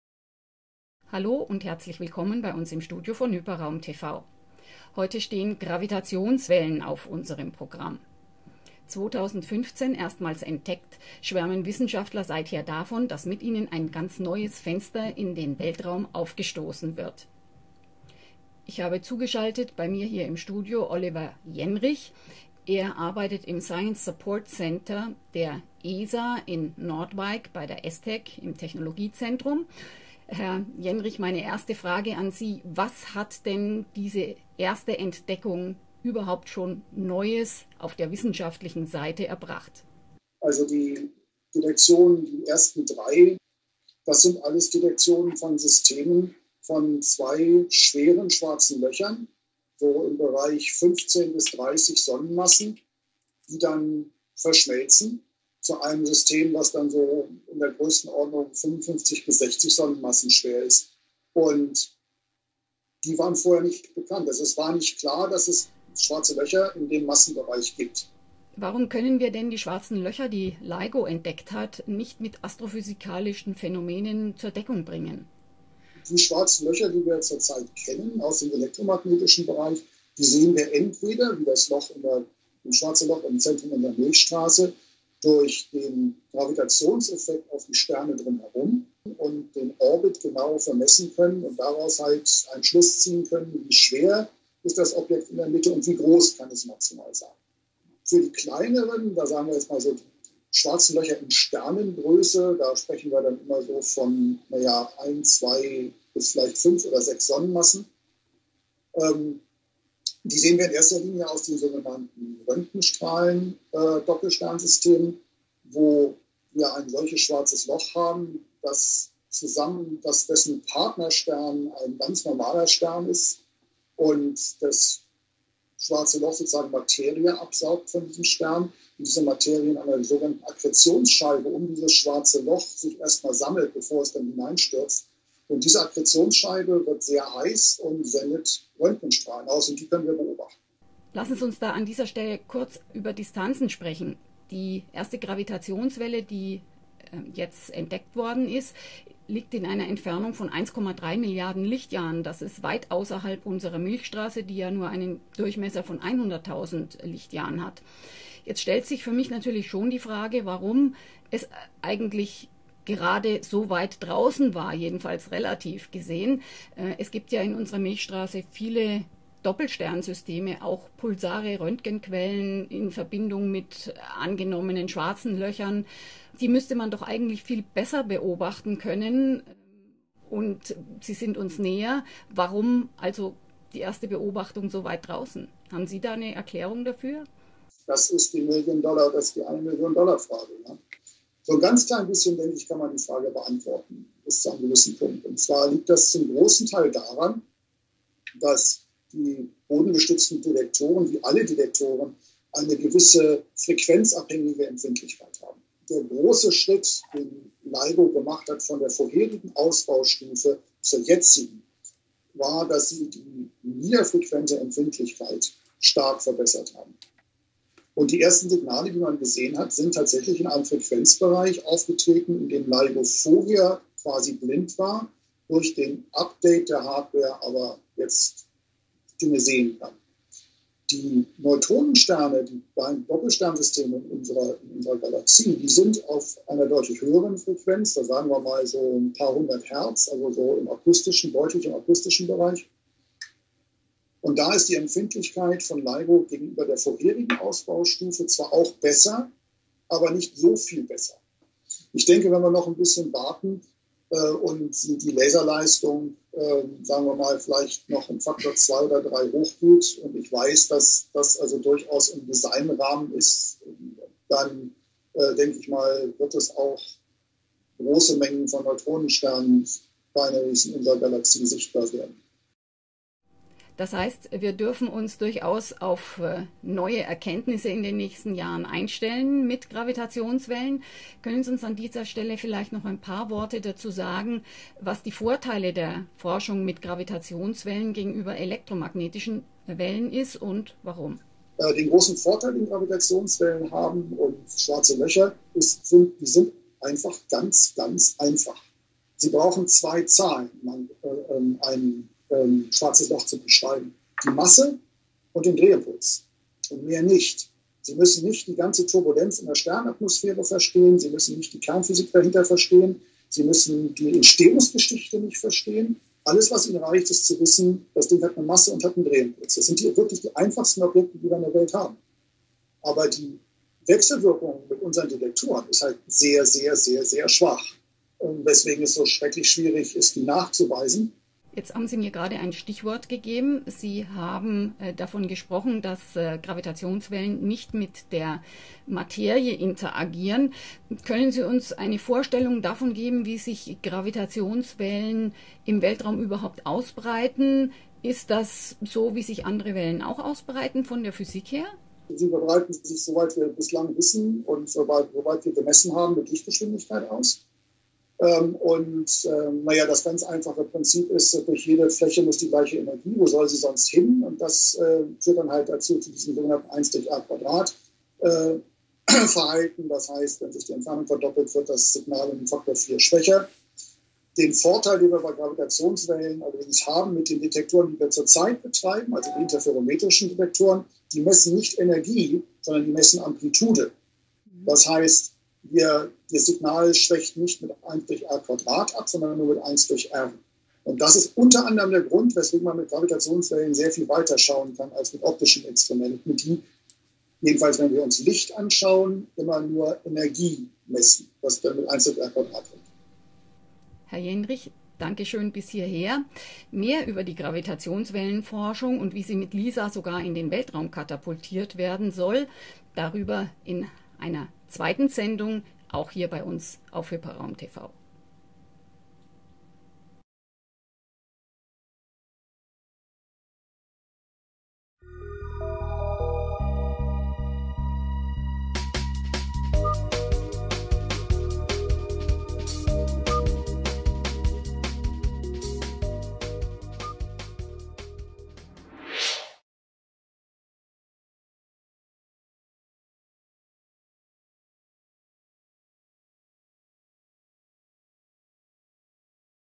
Gravitationswellen: Ursprung unbekannt (Skype-Talk Teil 1)
Skype-Talk via Skype: Mit Dr. Oliver Jennrich vom Science Support Office der ESTEC spricht Susanne Päch über den Nachweis von Gravitationswellen, welche Erkenntnisse sie uns bisher gebracht haben und warum sich die gemessenen Gravitationswellen bisher nicht mit beobachtbaren Himmelsphänomenen zur Deckung bringen lassen konnten. Hinweis in eigener Sache: Leider war der Ton des Skype-Interviews nicht optimal, sorry dafür.

Eine Sendung von und mit Susanne Päch
Eine Produktion von HYPERRAUM.TV
© Vorschaubild: AEI/ITP/ZIB